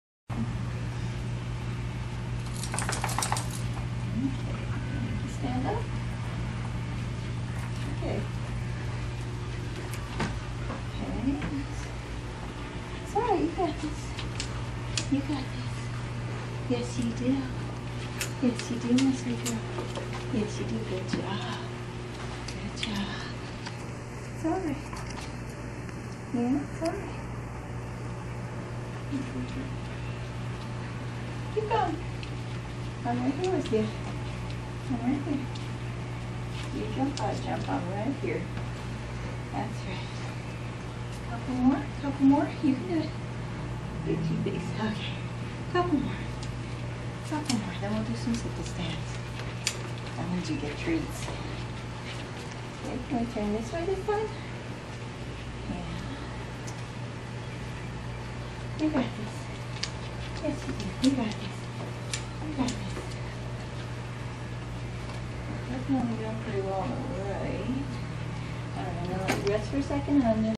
Zoey works the Obstacle Course
06/18/2016. Zoey works on walking the obstacle course. The sticks have been raised to position 2. Though still doing some knuckling she shows amazing progress in moving her legs and putting weight on her feet.